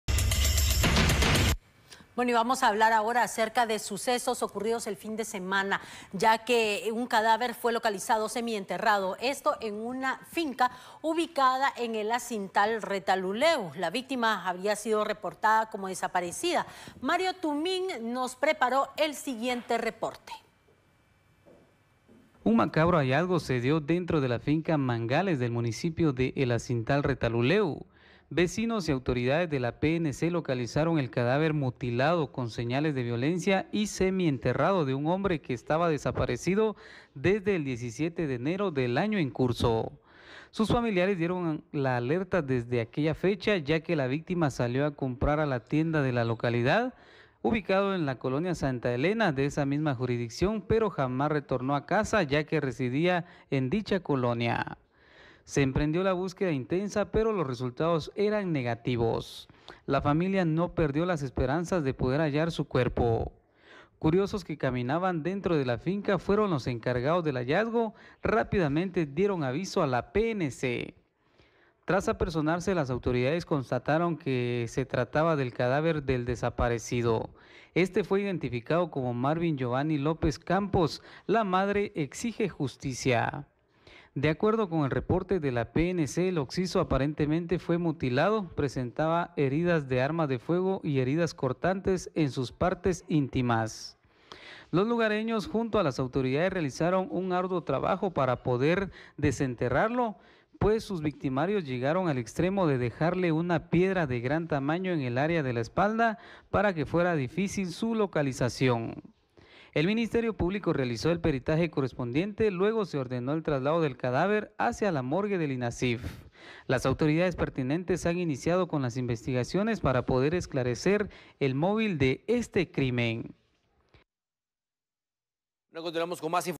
Localizan cadáver semienterrado en El Asintal
Visita nuestro sitio El cuerpo presentaba señales de violencia

Autoridades localizaron el cadáver de una persona semienterrado en una finca ubicada en El Asintal, Retalhuleu.